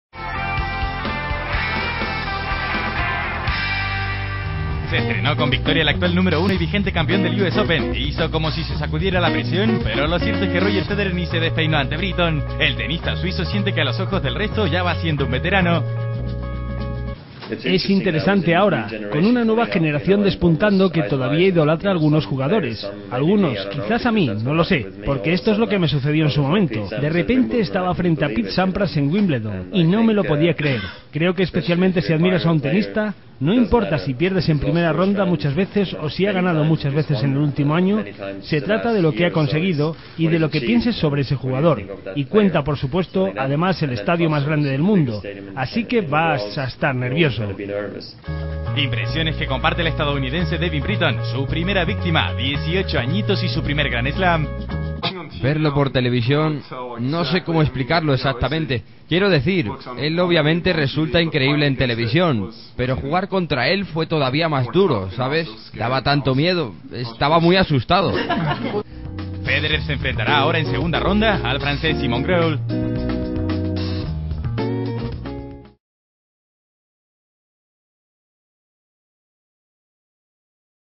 Federer debuta con victoria en el US Open - as.com
El número uno del mundo amplió a 35 su relación de victorias en el Abierto de Estados Unidos tras su trámite ante el joven Devin Britton, fijado en el puesto 1.340 por la ATP. El suizo busca en Nueva York ser el primer hombre desde el legendario Bill Tilden en 1920 en lograr seis títulos consecutivos en el cuarto Grand Slam. Ya ha avanzado algo. Britton, de dieciocho años, invitado y que disputaba su segundo partido de alta competición, fue capaz de apuntarse un ''break'' en el segundo set.